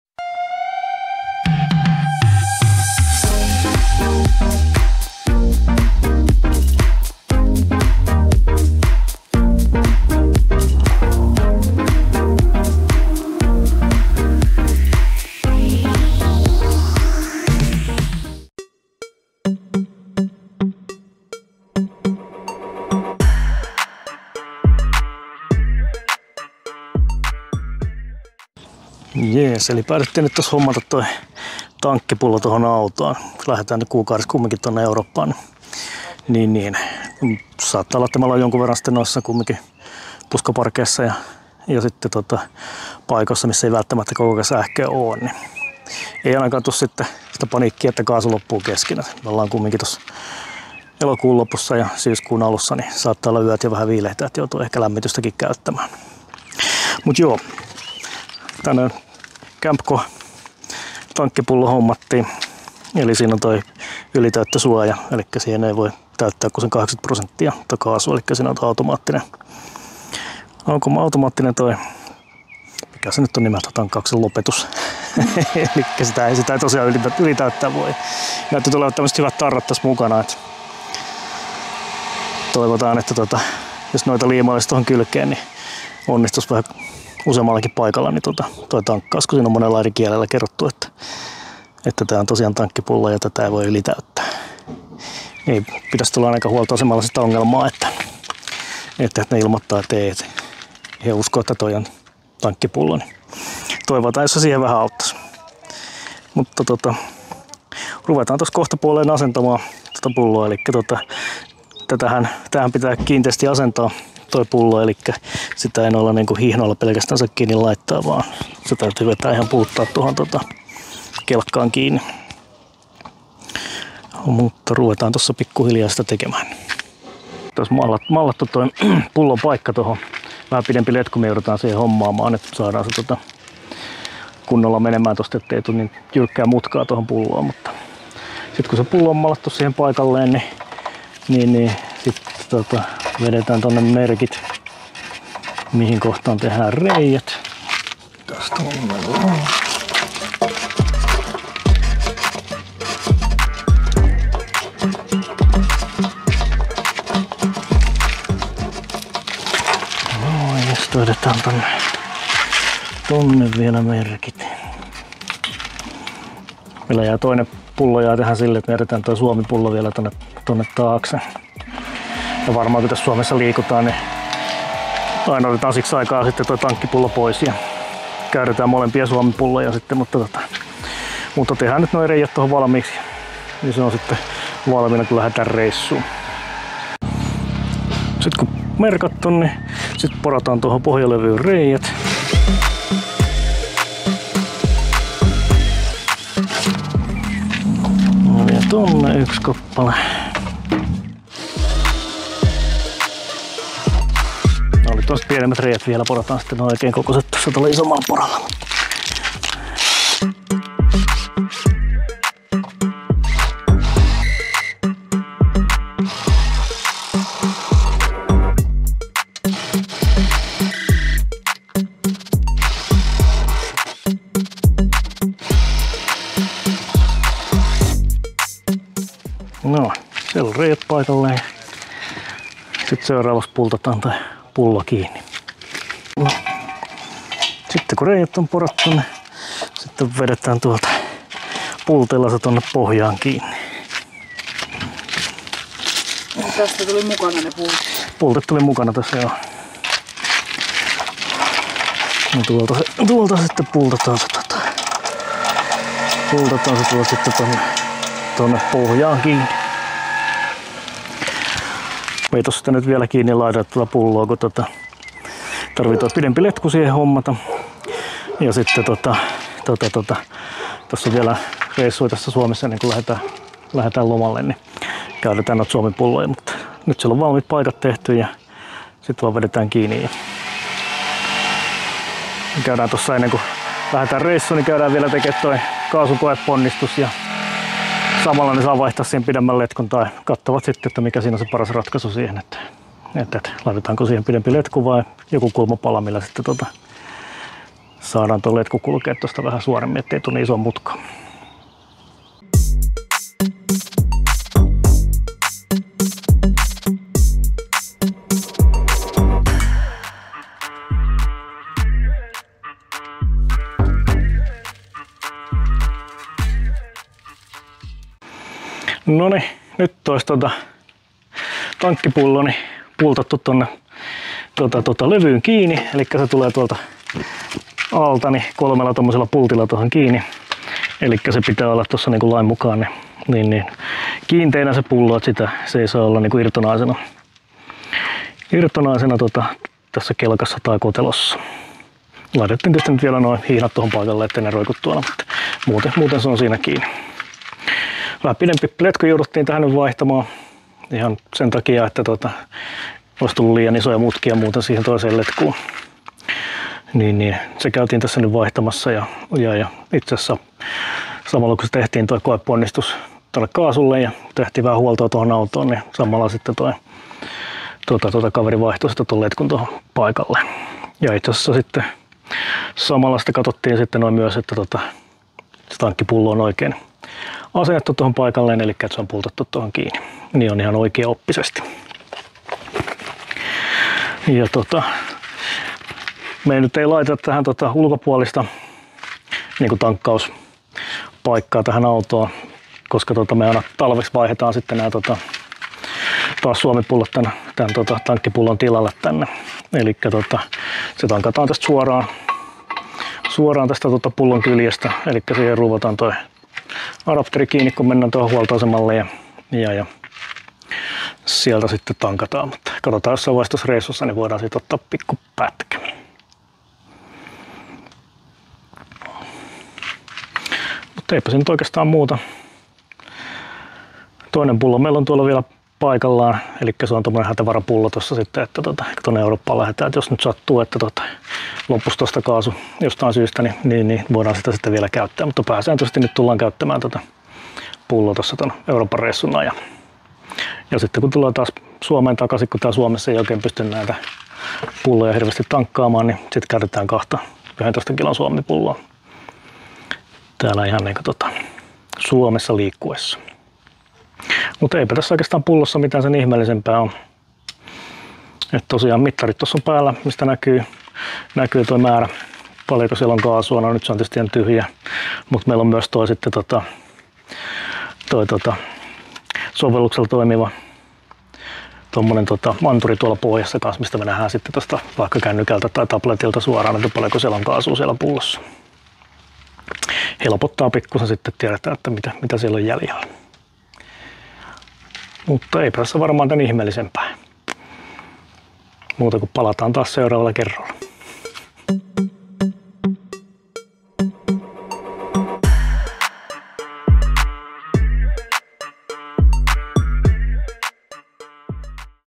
Tankkipullo kaasulle matkailuautoon
Päätimme hommata tankkipullon kaasulle tulevaa Euroopan reissua ja muita ulkomaan reissuja silmällä pitäen. Videolla hieman tietoa tankkipullon asennukseen liittyen.
Video ei ole tehty yhteistyössä kenenkään kanssa 😂